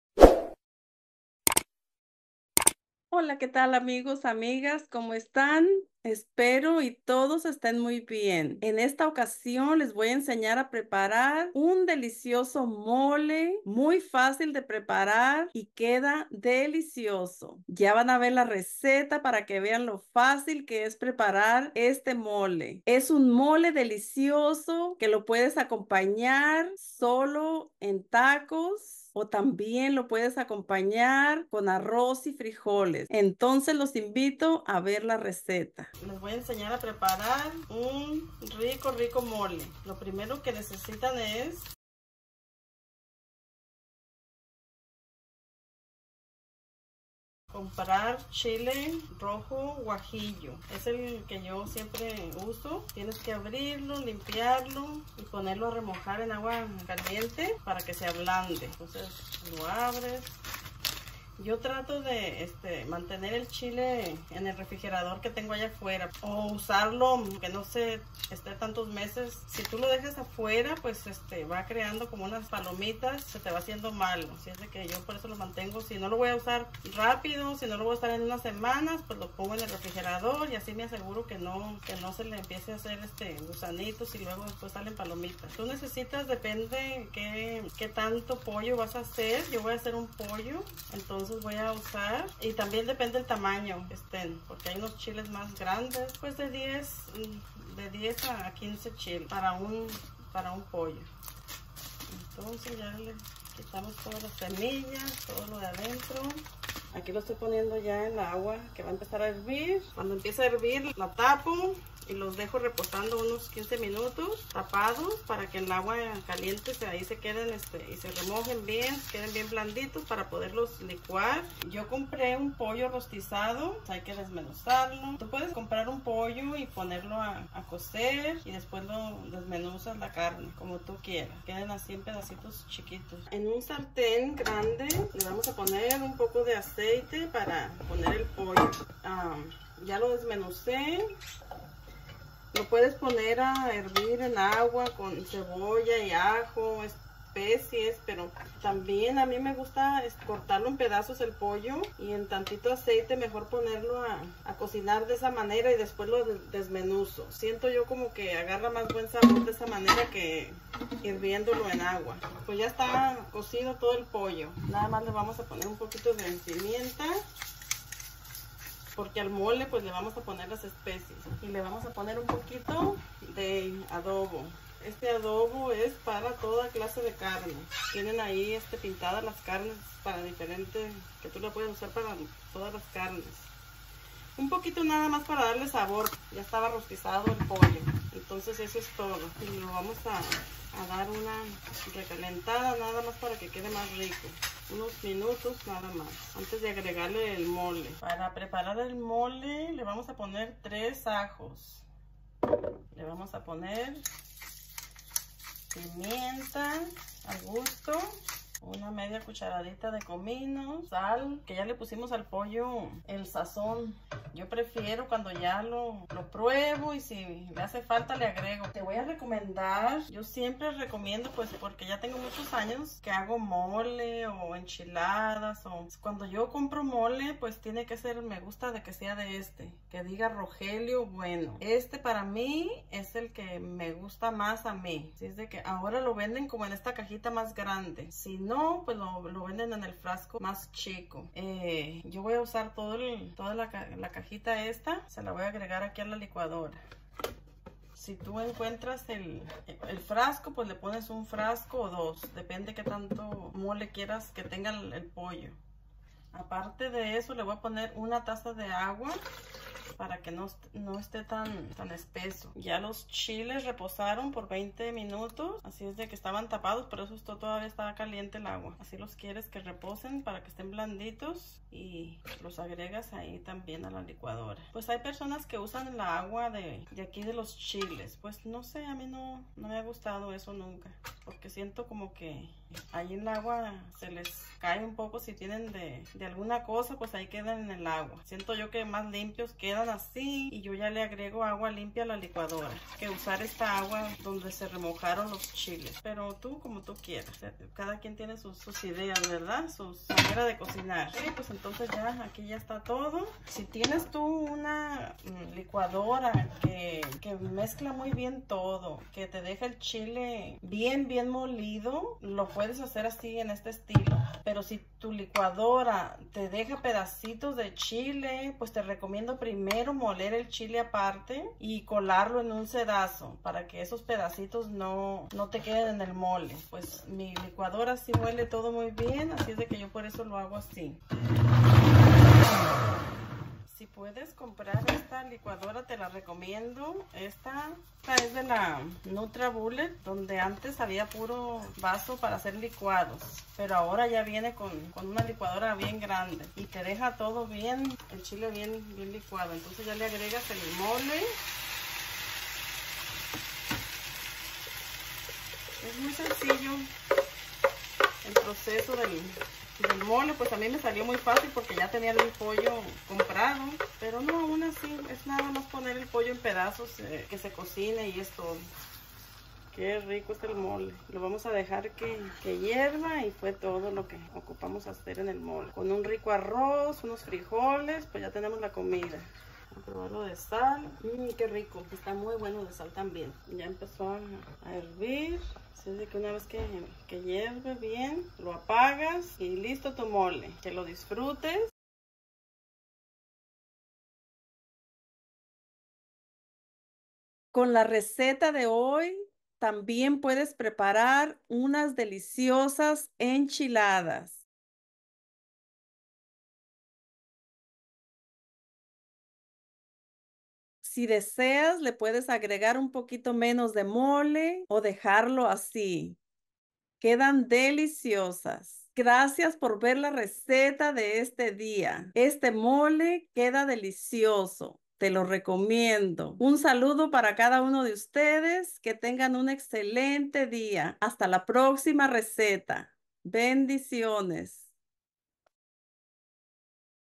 MOLE ROJO DELICIOSO Y MUY FACIL DE PREPARAR
MOLE ROJO

INGREDIENTES
12 CHILES GUAJILLO
1 POLLO ROSTIZADO
ACEITE DE AGUACATE
PIMIENTA AL GUSTO
ADOBO GOYA O SAZON AL GUSTO
3 AJOS GRANDES
1/2 CUCH DE COMINO MOLIDO
SAL AL GUSTO
1 CAJA DE MOLE ROGELIO BUENO
1 TAZA DE AGUA